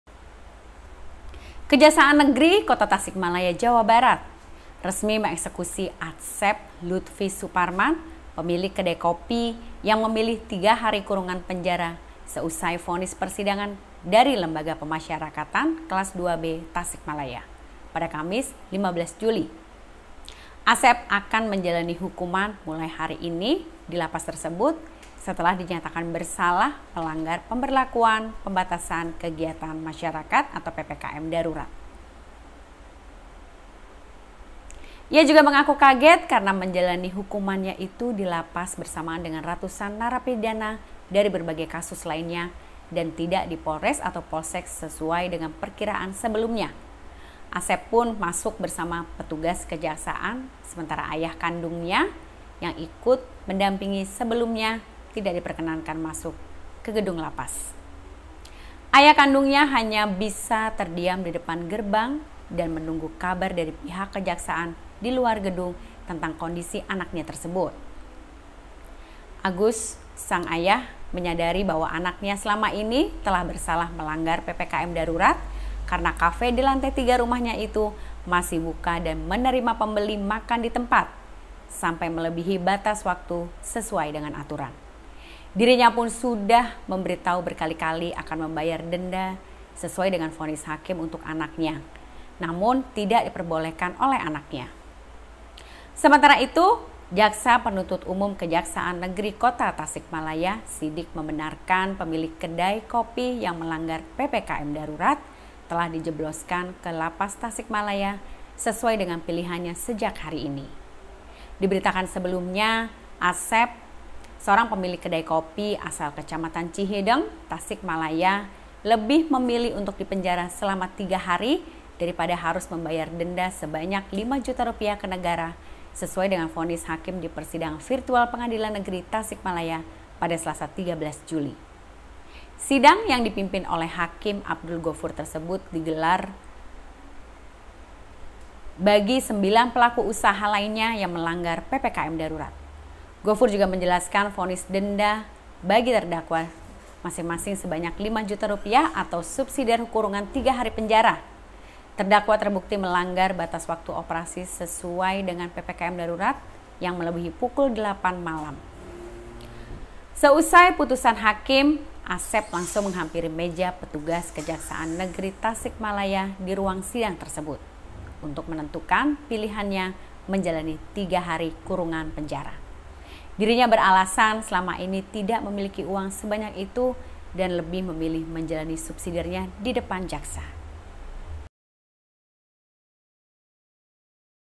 Asep Pemilik Kedai Kopi di Tasikmalaya Pelanggar PPKM Ditahan di Lapas
Asep Lutfi Suparman (23), pemilik kedai kopi di Tasikmalaya, mengaku kaget dirinya dibawa ke Lembaga Pemasyarakatan (Lapas) Kelas II B. Awalnya, kata Asep, dirinya akan ditahan selama tiga hari di penjara kantor polisi. "Saya kaget, ya kaget. Saya kira ditahannya di Polsek atau Polres, tapi ternyata saya ditahannya di Lapas. Tapi saya siap," jelas Asep kepada wartawan saat hendak memasuki gerbang besi Lapas, Kamis siang. Asep yang divonis bersalah karena melanggar PPKM darurat itu sebelumnya memilih dipenjara daripada membayar denda Rp 5 juta.